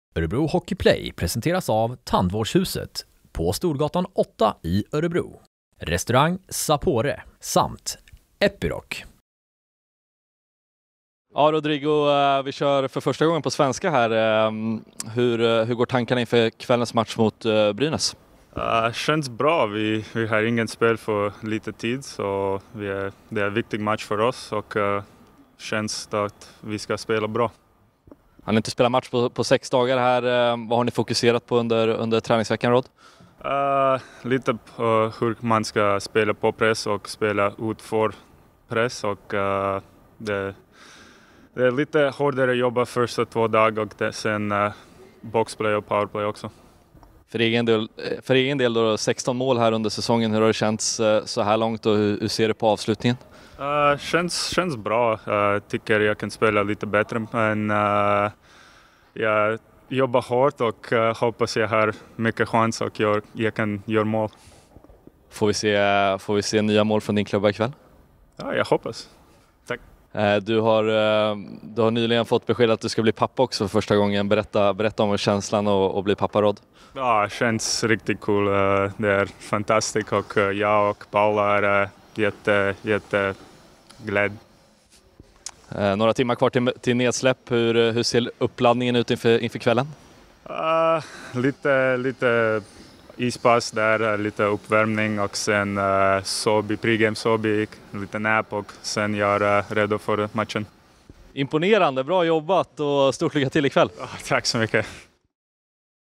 "En viktig match för oss" - Rodrigo Abols snackar upp kvällens match - På svenska!
Rodrigo Abols snackar upp kvällens hemmamatch mot Brynäs - På svenska!